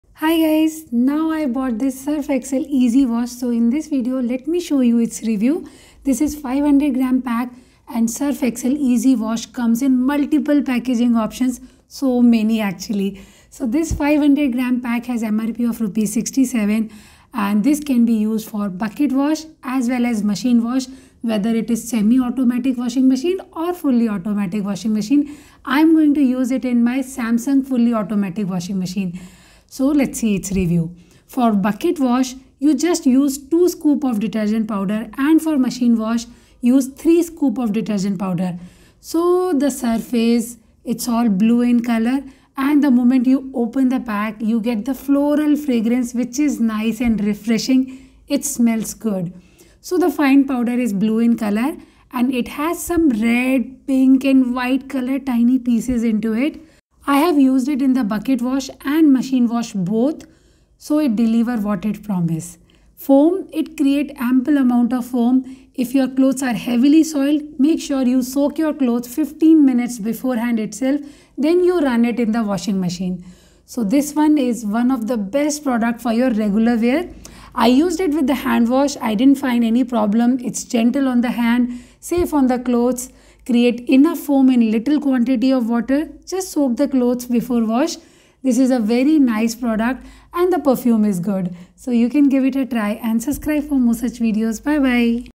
"Surf Excel Easy Wash Review and Demonstration" | How to use Surf Excel Easy Wash
Surf Excel Easy Wash Detergent Powder

Chapters:
0:00: Introduction
06:16: Pack details of Surf Excel Easy Wash
30:17: Color, Dosage of Surf Excel Easy Wash
01:00:23: How to use Surf Excel Easy Wash  in Bucket wash
1:13:02: How to use Surf Excel Easy Wash  in Washing machine
1:27:00: Final Conclusion about performance of Surf Excel Easy Wash